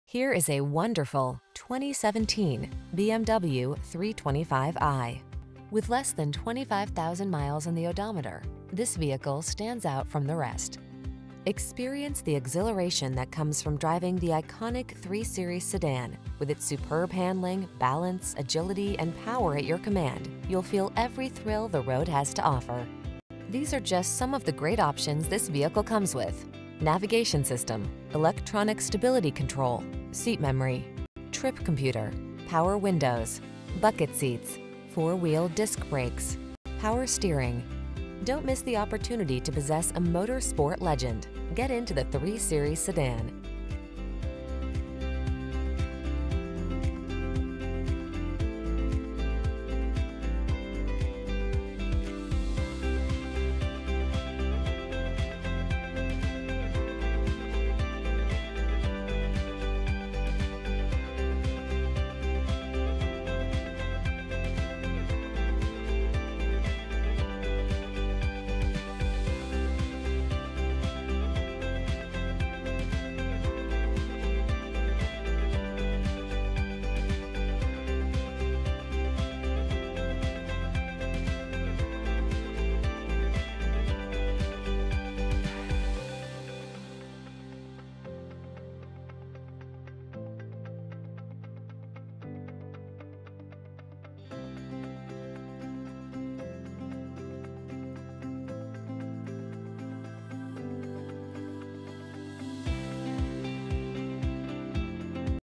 2017 BMW 3 Series Tuscaloosa, Birmingham, Greensboro, Bessemer, AL, Columbus, MS P156308
White Metallic Used 2017 BMW 3 Series available in Tuscaloosa, Alabama at BMW of Tuscaloosa. Servicing the Birmingham, Greensboro, Bessemer, Columbus, MS area.
2017 BMW 3 Series 330e iPerformance - Stock#: P156308 - VIN#: WBA8E1C31HA156308

Priced below KBB Fair Purchase Price! Odometer is 1286 miles below market average! Clean CARFAX.